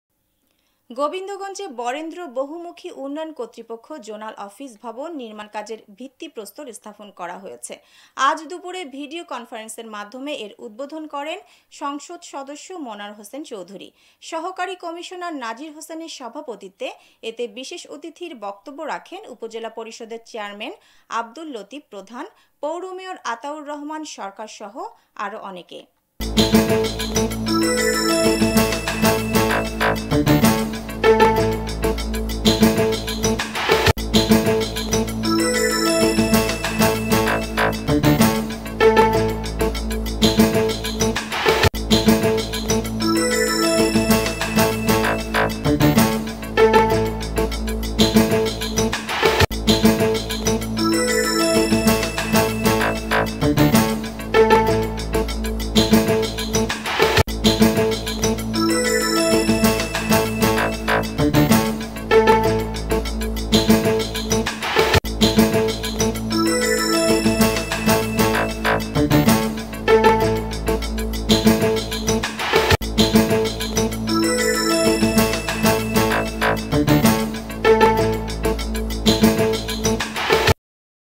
বরেন্দ্র বহুমুখী উন্নয়ন কর্তৃপক্ষ জোনাল ভবন নির্মাণ কাজের ভিত্তি প্রস্তর স্থাপন । Radio Sarabela
গোবিন্দগঞ্জে বরেন্দ্র বহুমুখী উন্নয়ন কর্তৃপক্ষ জোনাল অফিস ভবন নির্মাণ কাজের ভিত্তি প্রস্তর স্থাপন করা হয়েছে। আজ (মঙ্গলবার) দুপুরে ভিডিও কনফারেন্সের মাধ্যমে এর উদ্বোধন করেন সংসদ সদস্য মনোয়ার হোসেন চৌধুরী। সহকারী কমিশনার নাজির হোসেনের সভাপতিত্বে এতে বিশেষ অতিথির বক্তব্য রাখেন উপজেলা পরিষদের চেয়ারম্যান আব্দুল লতিফ প্রধান, পৌর মেয়র আতাউর রহমান সরকার আরও অনেকে।